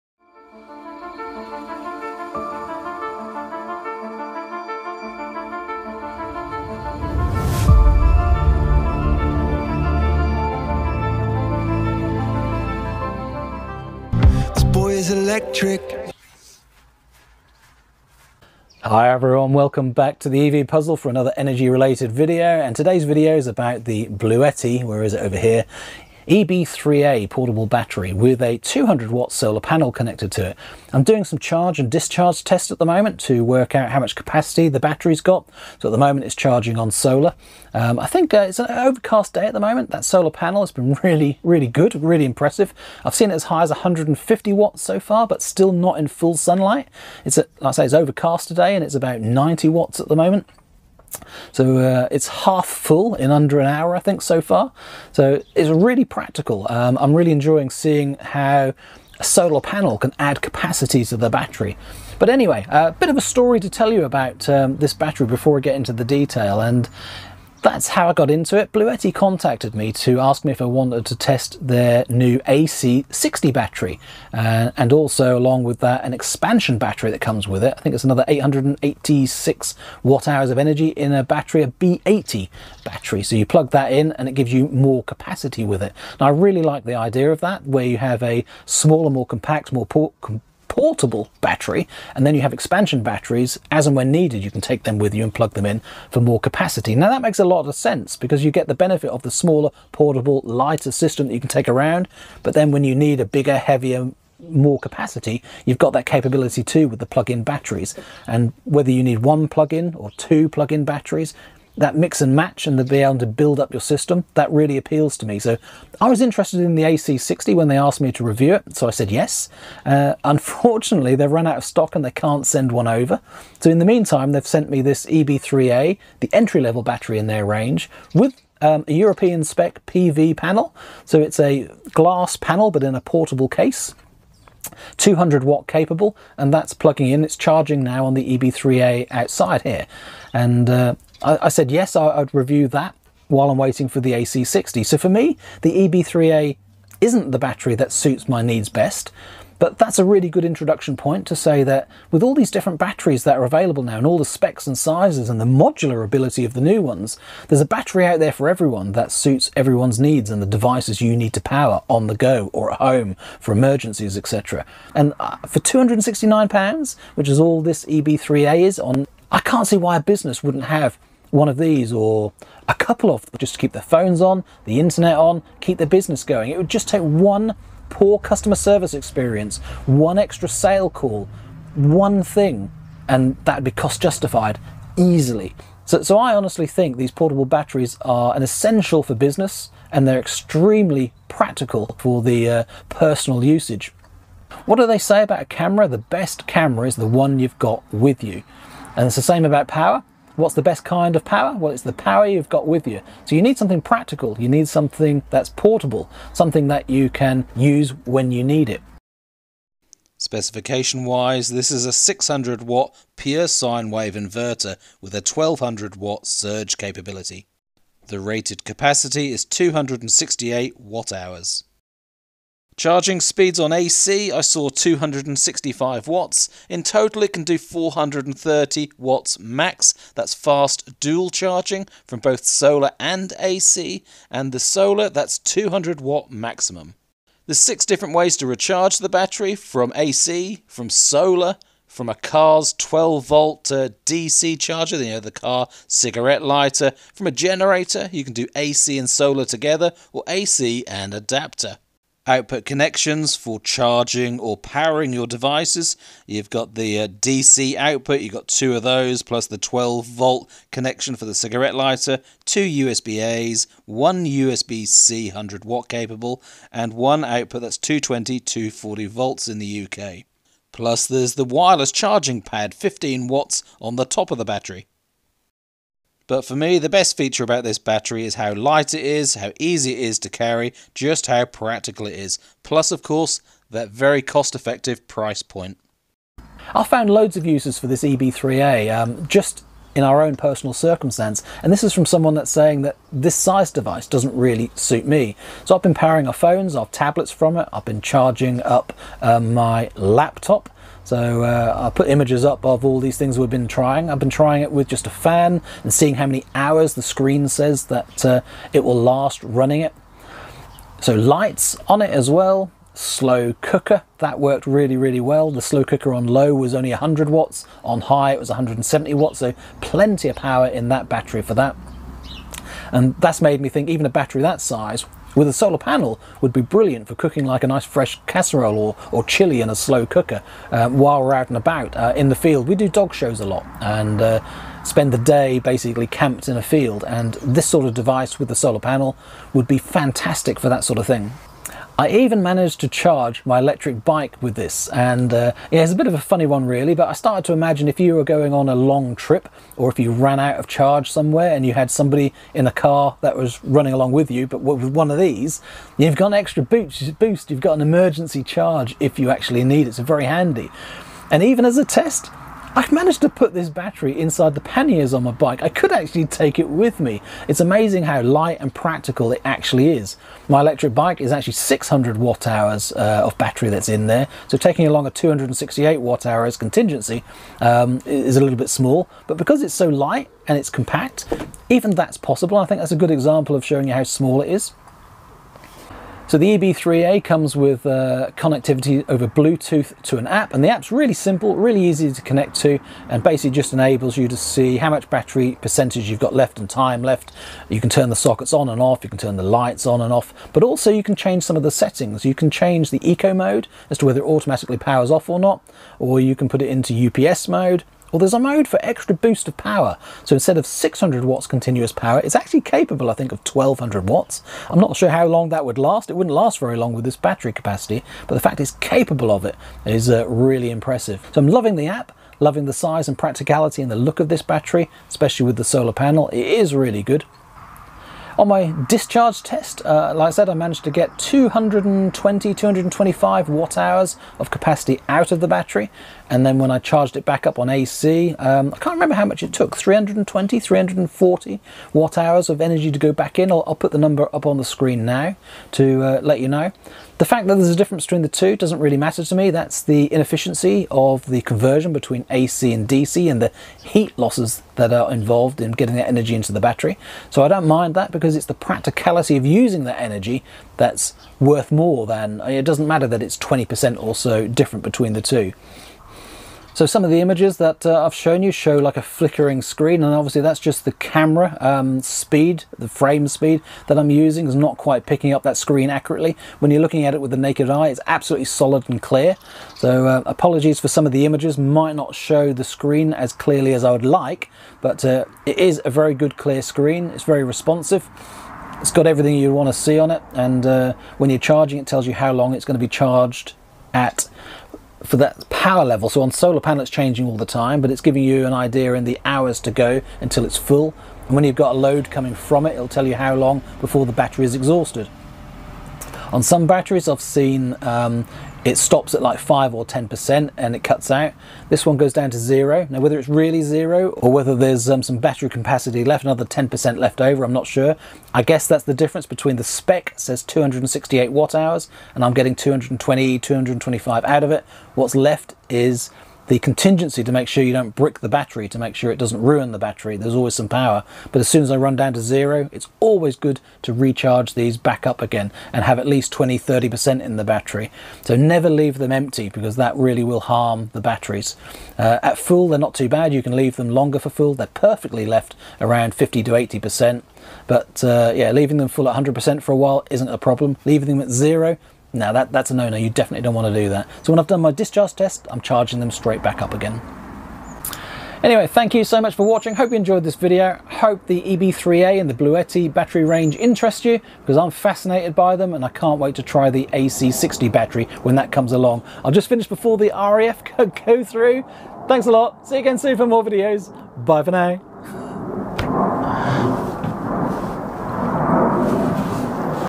Testing the Bluetti EB3A portable power station/battery with 200w PV panel
Brilliant entry level portable power station with a host of uses and more than decent output power capability. It's a little short on battery capacity that that can be multiplied 3-4 times using a solar PV panel in the right light conditions.

Overall this is a light and practical power station that will keep you charged up wherever you go.